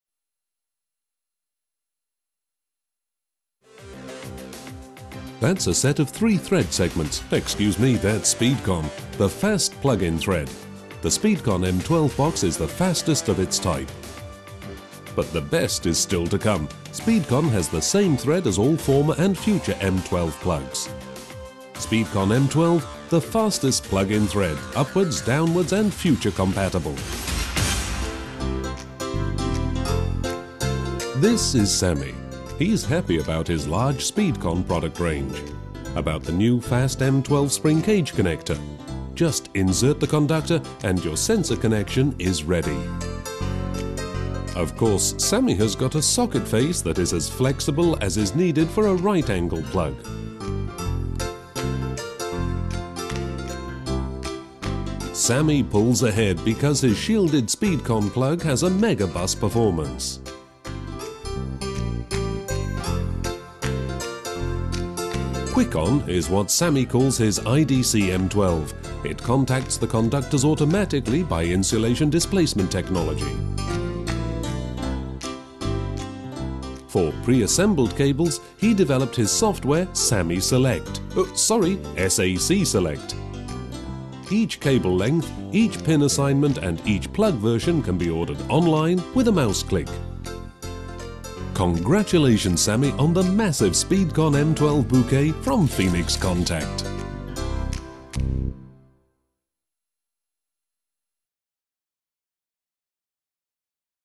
Phoenix Contact SPEEDCON M12 Connectors
Product animation for Phoenix Contact's SPEEDCON line of M12 connectors.  SPEEDCON's revolutionary M12 connection system makes a secure, water tight, vibration-proof connection between cordsets and devices with a simple half-turn. Best of all, SPEEDCON works with all existing M12 connectors so it's backwards, forwards and future compatible.